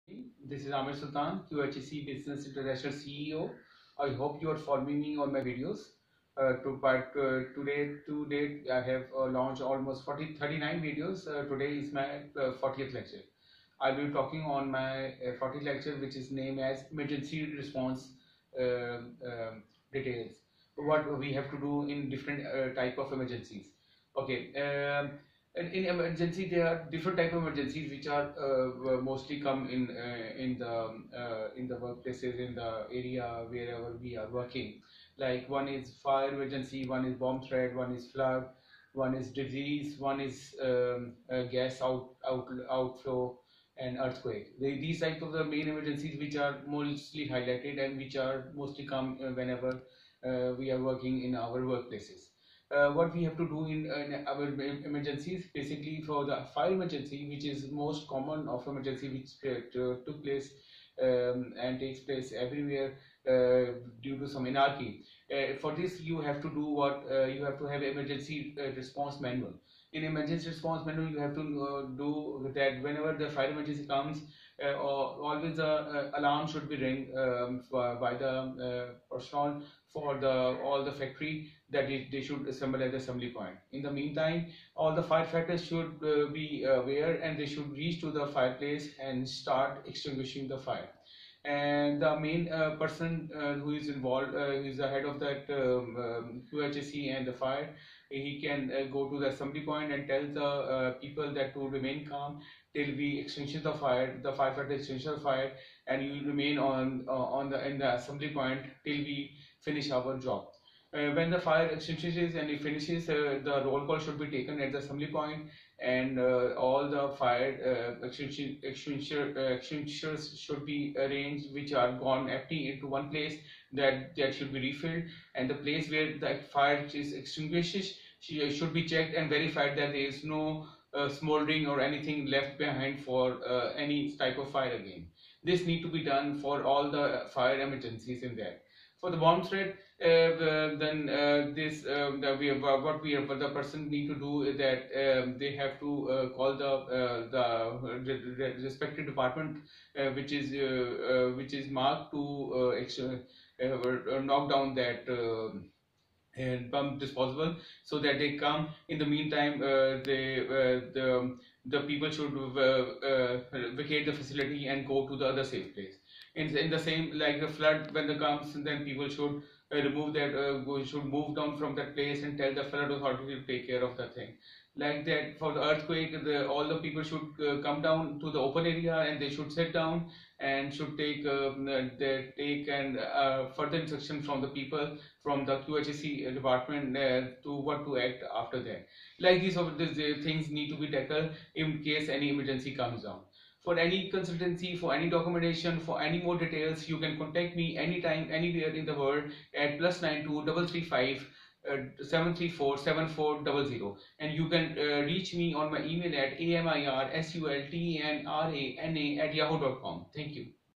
Emergency response manual
Emergency response manual plays a very important role in an organization. Whenever any anarchy occurs the emergency response manual guidelines helps in settling it. There are many types of emergencies present. For each of the emergency different acts are required to subside them.